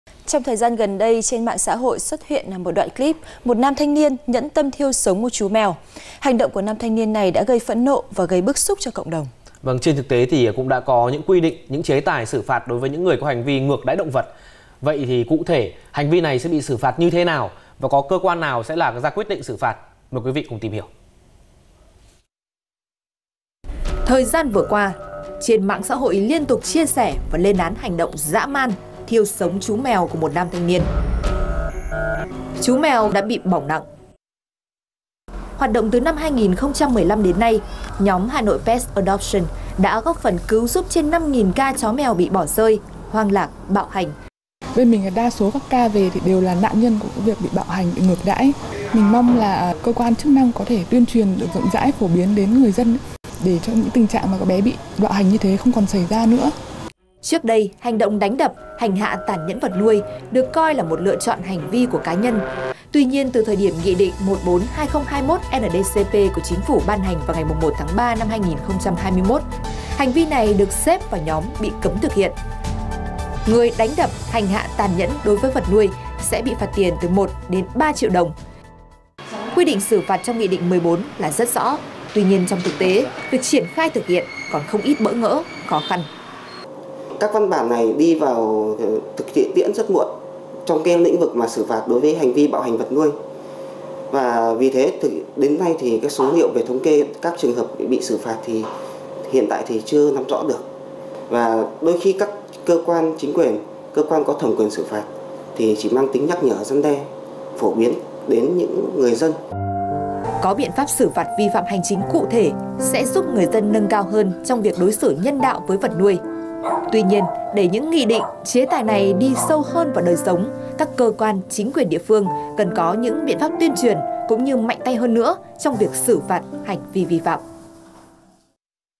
Cần xử phạt nghiêm hành vi bạo hành vật nuôi
Chó mèo là loài động vật nuôi không có khả năng tự bảo vệ, phụ thuộc rất nhiều vào chủ, và do đó có nhiều người vẫn xem chó mèo là tài sản chứ không phải là một cá thể sống và tự cho mình quyền hành hạ, ngược đãi những con vật này. Đây là hành vi tàn nhẫn, làm xã hội phẫn nộ và đáng bị lên án.